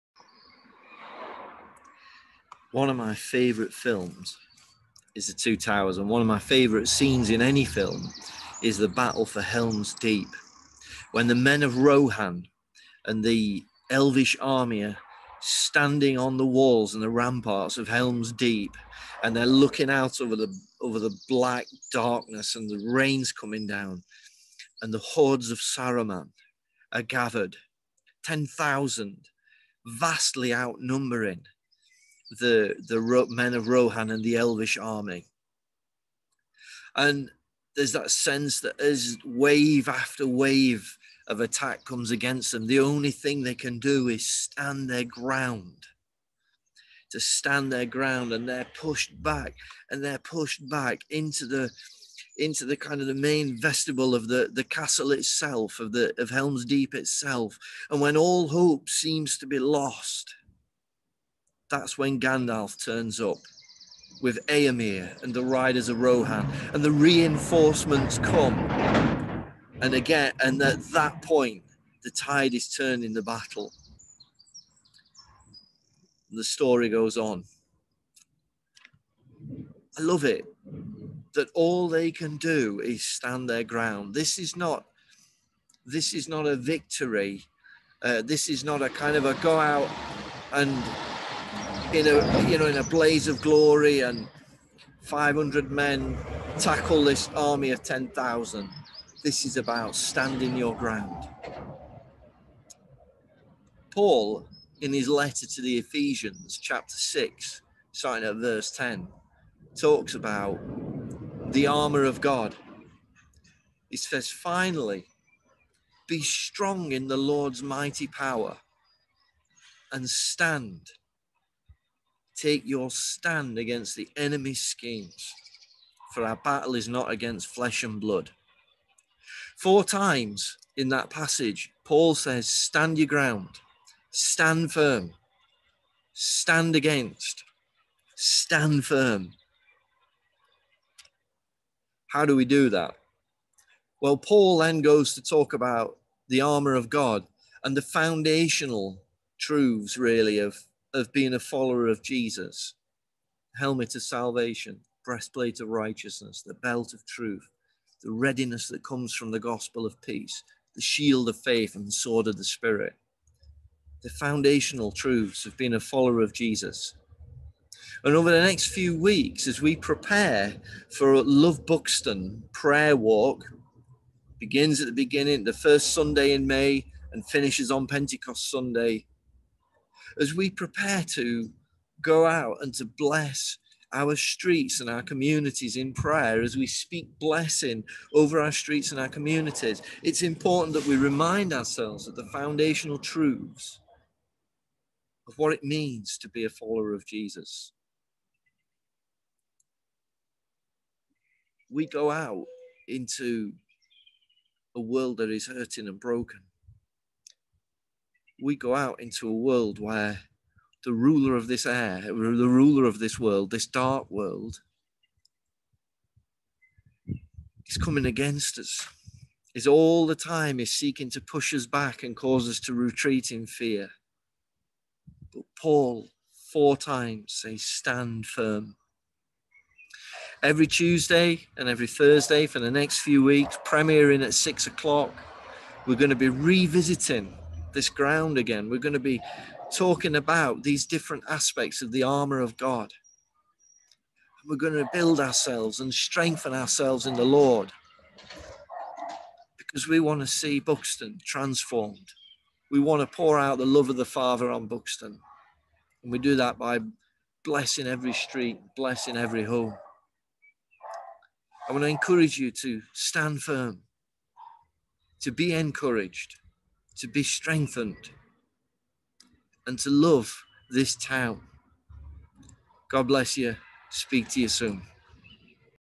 'Stand Firm' - The Armour of God Session 1
This is part 1 of a new series on the Armour of God, that we shall be exploring as a preparation for 'Love Buxton: Prayer Walk'. These are foundational truths that enable us to 'Stand firm' in God's mighty power.

Love Buxton: Prayer Walk shall be from Sunday 2nd May to Sunday 23rd May - Pentecost. We shall be encouraging followers of Jesus to walk down their streets, speaking out blessings over their neighbors and businesses.